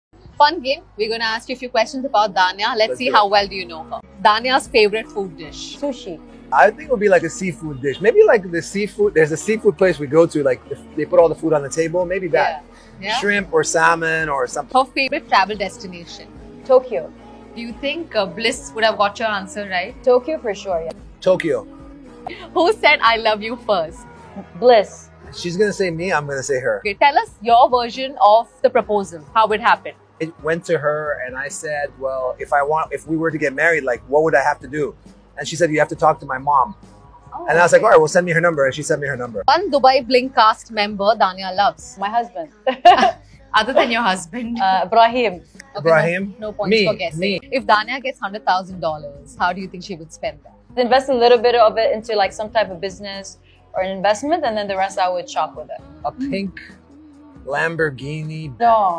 Dj Bliss Plays A Fun Game On How Well He Knows His Wife | Curly Tales ME
Watch How Well DJ Bliss Knows His Wife Danya By Answering Some Fun Questions ❤️😉